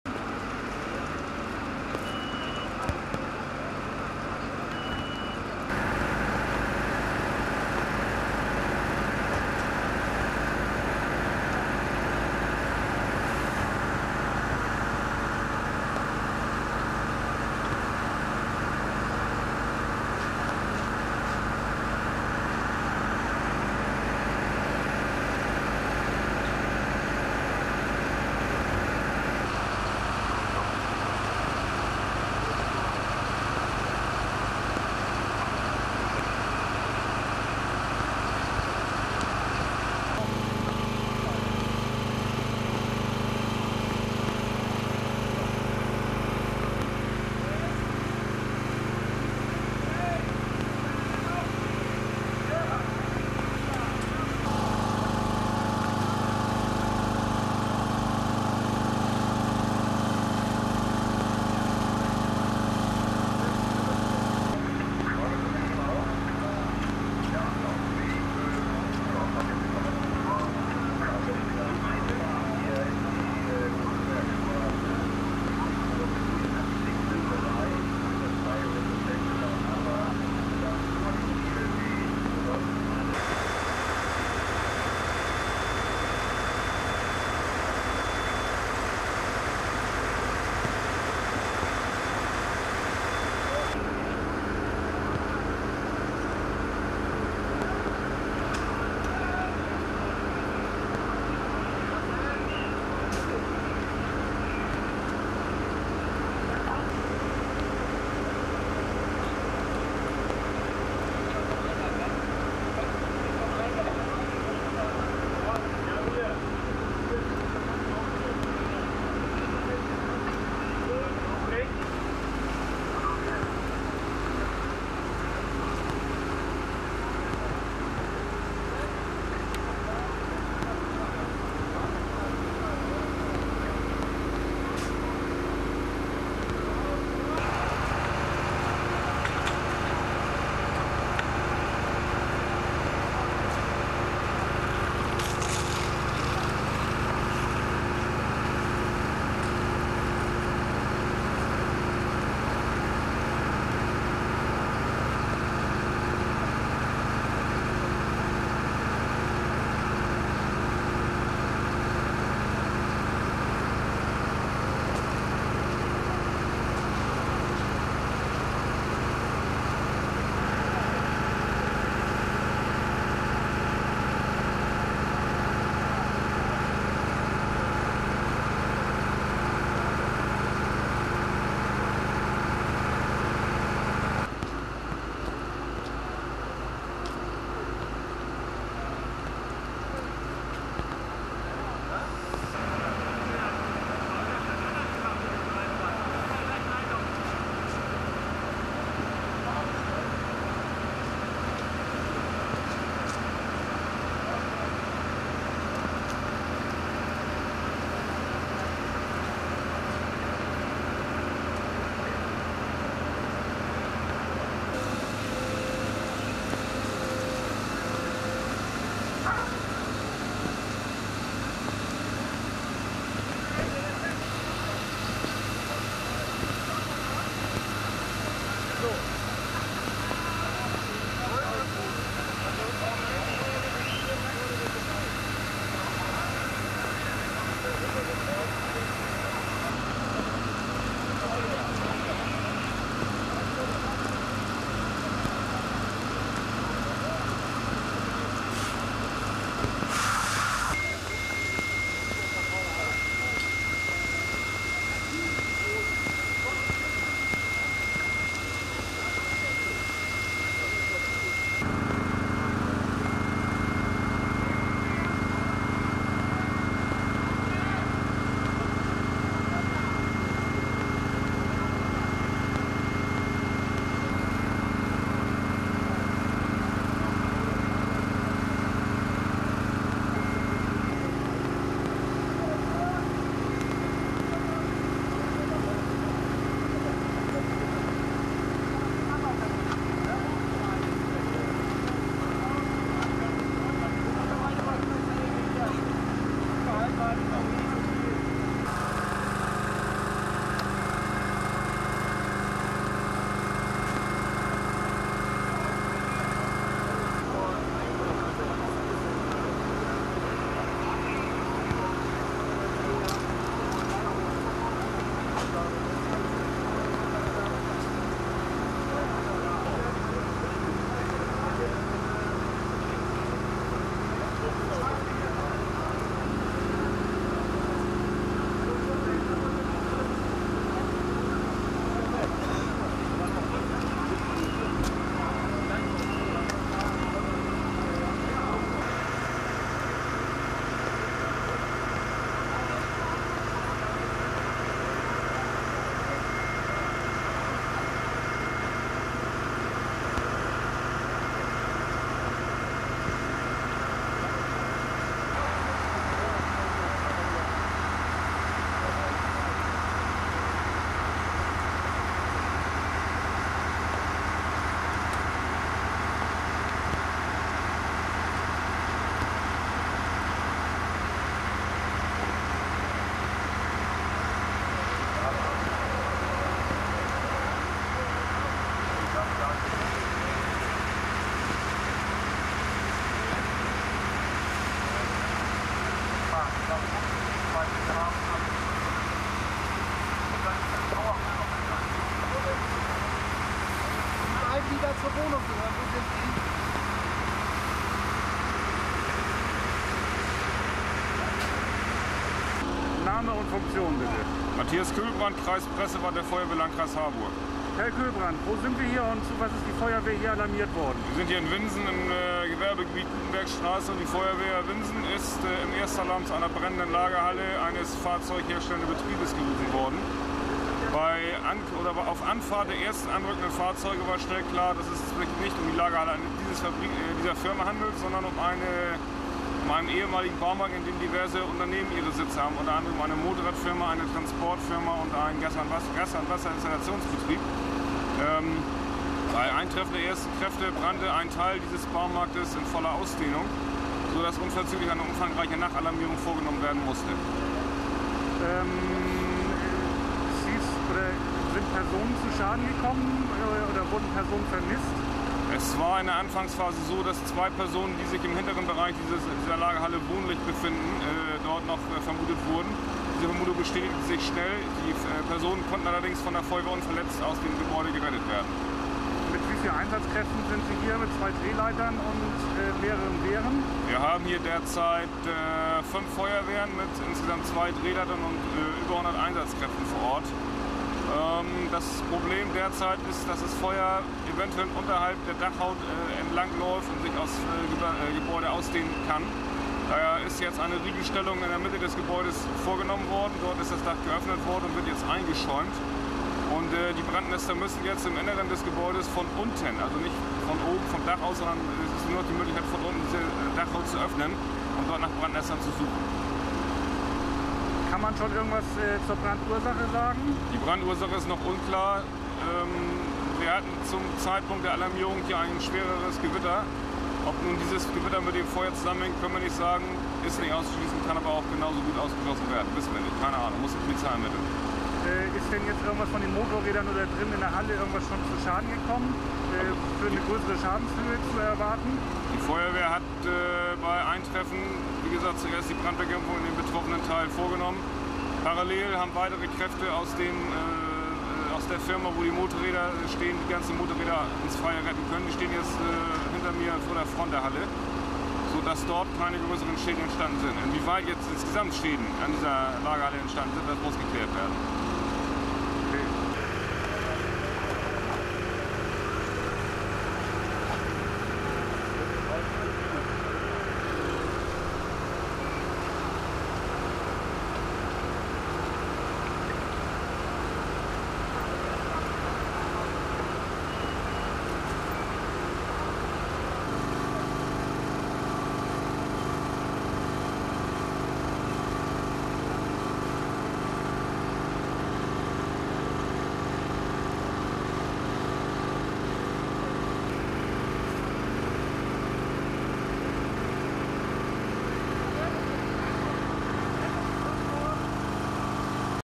Feuer nach Blitzschlag?
Blitzschlag im ehemaligen Baumarkt - 2 Verletzte
Feuerwehr kann teure Bikes aus einem Moto - Shop retten

Winsen / Kreis Harburg DOK ID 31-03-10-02 | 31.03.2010, 01:00 Uhr

Starker Regen und ein heftiges Gewitter haben für zahlreiche Einsätze der Feuerwehren in Hamburg und im Umland gesorgt. In Winsen brach nach einem heftigen Gewitter ein Feuer in einen ehemaligen Baumarkt aus. Zwei Menschen konnten noch rechtzeitig gerettet werden. Mehr als 100 Feuerwehrleute waren im Einsatz. Das Feuer brach in einem Motorradshop aus. Die Feuerwehr konnte noch zahlreiche Maschinen retten und rausschieben. Die Löscharbeiten waren schwierig, überall bildeten sich Brandnester in der Decke. Die Löscharbeiten gingen bis in den frühen Morgen.